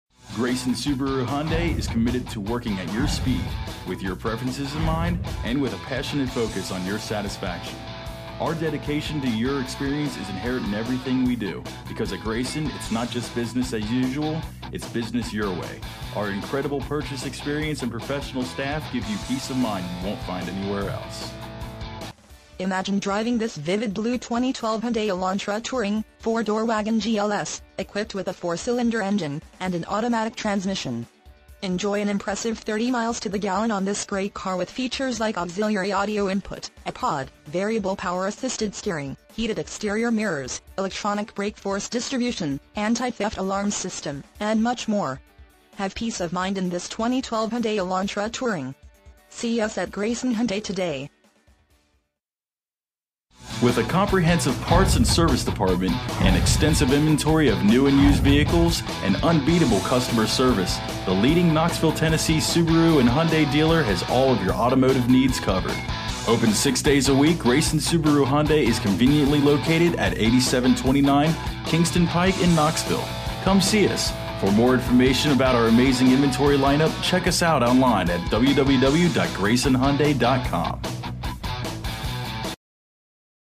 2012 Hyundai Elantra Touring Grayson Hyundai Knoxville, TN 37923
Hyundai Elantra Touring Imagine driving this Vivid Blue Hyundai Elantra Touring 4 Dr Wagon, equipped with a 4 Cylinder engine and an automatic transmission. Enjoy an impressive 30 miles to the gallon on this great car with features like Auxiliary Audio Input - Ipod, Variable Power-Assisted Steering, Heated Exterior Mirrors, Electronic Brakeforce Distribution, Anti-Theft Alarm System, Emergency Braking Assist, 4 Way Power Driver Seat, Xm Satellite Radio, and much more. Have peace of mind in this Hyundai Elantra Touring. See us at Grayson Hyundai today!
MILES: 42,134
VIN:KMHDB8AE5CU128179

Grayson Hyundai
8729 Kingston Pike
Knoxville, Tennessee 37923